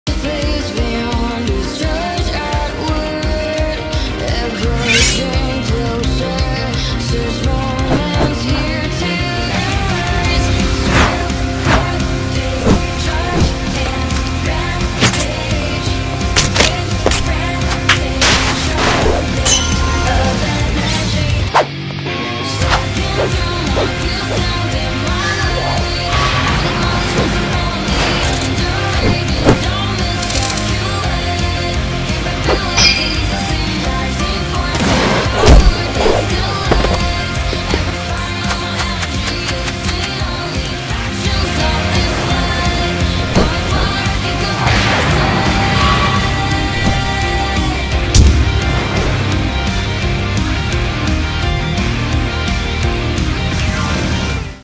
Youmu VS Reimu (Touhou Animation PCBirthday)
In celebration of Perfect Cherry Blossom's 20th year since release, I have made an animation to commemorate it with Youmu Vs Reimu